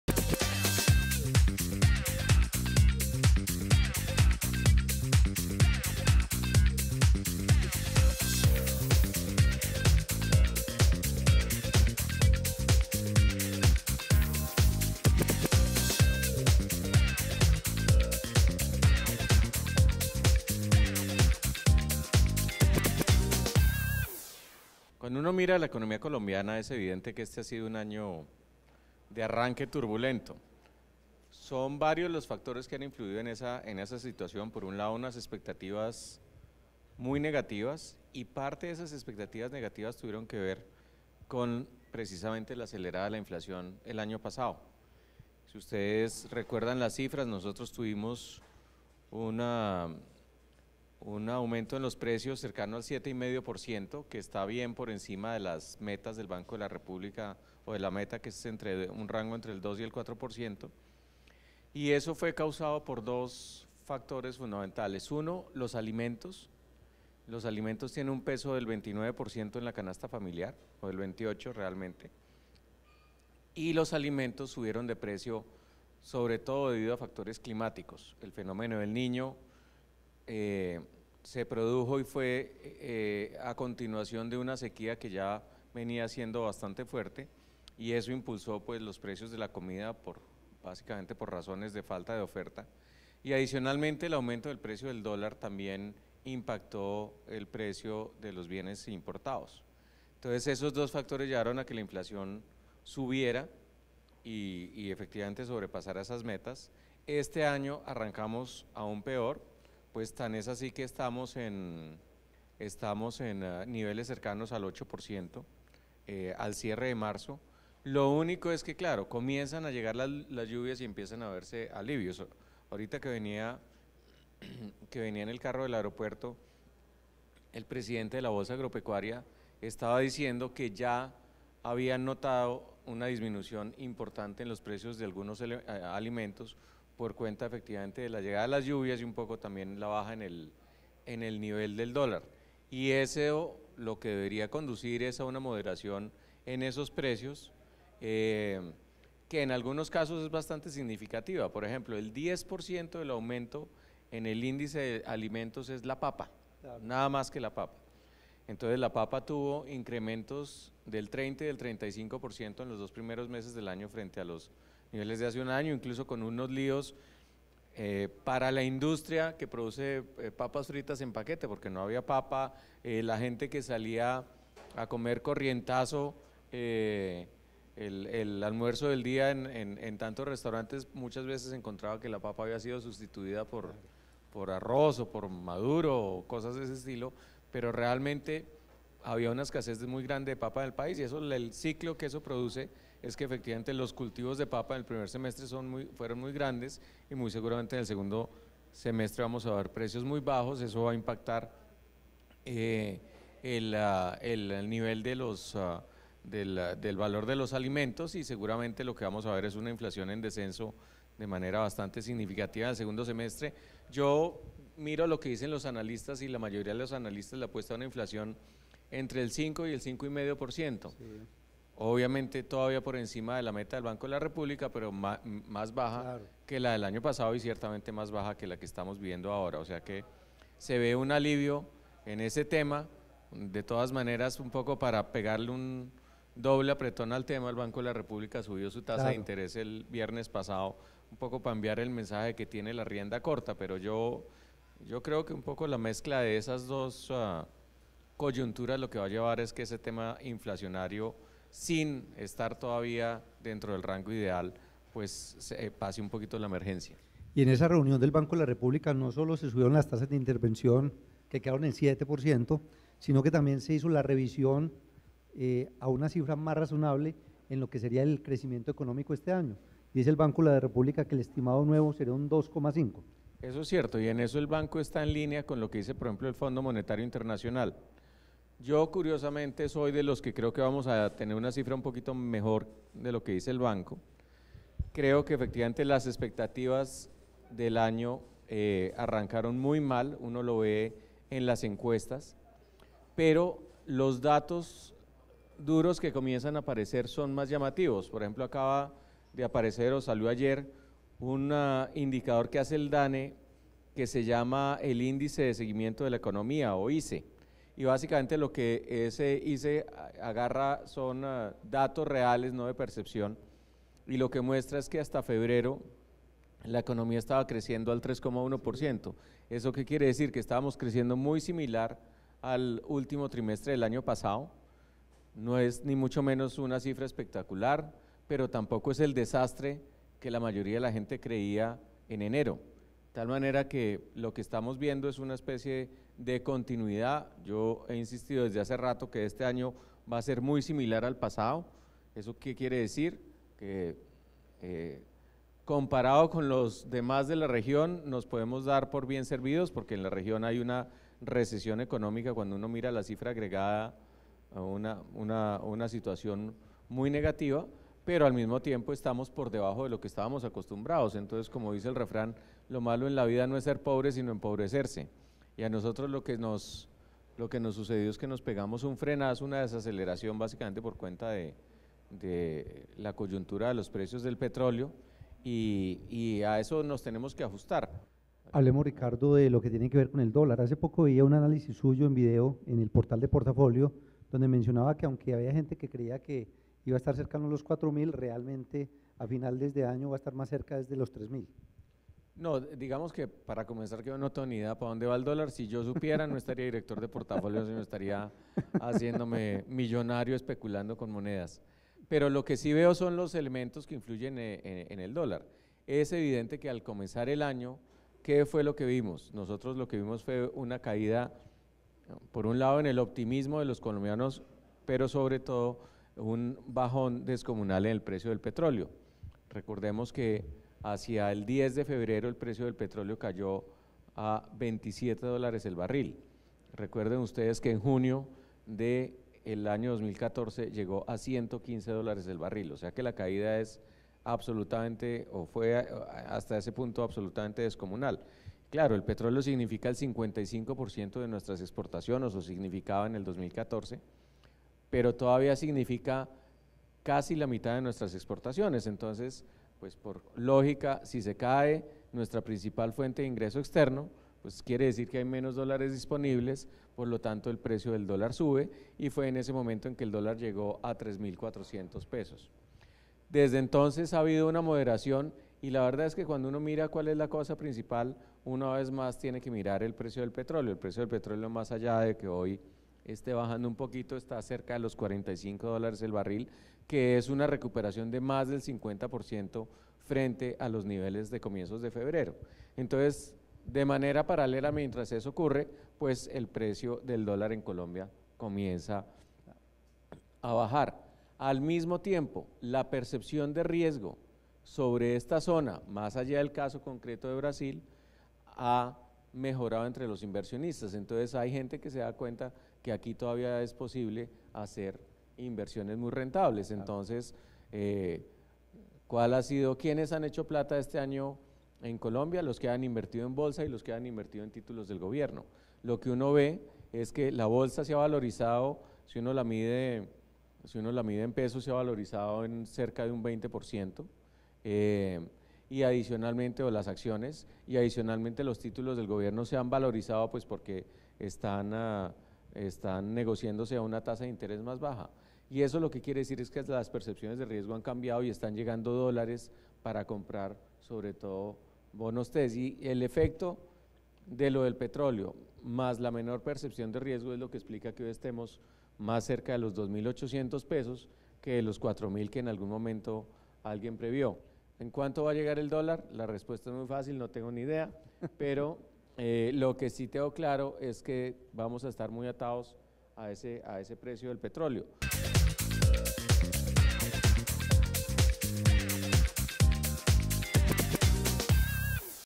Análisis económico de @Ravilapinto, director de @Portafolioco - [Negocios en Tm] ®
El director del diario Portafolio, Ricardo Ávila (Twitter: @Ravilapinto) dialoga con nuestro director Juan Carlos Yepes (Twitter: @Juancarlosy) sobre las principales cifras económicas de Colombia y el mundo.

“2016 ha sido un año de arranque turbulento para la economía colombiana. Comenzó con expectativas negativas como consecuencia de la aceleración de la inflación del año pasado, que contó con un aumento en los precios cercano al 7.5%, debido al impacto que tuvo el fenómeno del niño en el costo de los alimentos y como consecuencia, generó la falta de oferta de estos productos. Este año estamos en niveles de inflación cercanos al 8% en el mes de marzo. Sin embargo, la llegada de las lluvias y la baja en el nivel de dólar, ya están generando una disminución en los precios de los alimentos y una nueva cifra en los niveles de inflación, la cual rondará entre el 5% y 5,5% a partir del segundo semestre del año.

Referente a la cifra del crecimiento económico para este año, estoy de acuerdo con el Indicador de Seguimiento Económico (ISE), del Departamento Administrativo Nacional de Estadística (DANE) publicado en días pasados, que señala que hasta el mes de febrero el nivel de crecimiento era de 3,1%”, expresó Ricardo Ávila, Director

De esta forma se dio apertura al conversatorio, organizado por Portafolio y que tuvo como objetivo resolver las dudas que se tienen frente a los temas que les interesan al país y Antioquia en economía y negocios, y lo que viene en los próximos meses para la región, en el que participó junto a Ávila, Juan Carlos Yepes, periodista, empresario y director del programa Negocios.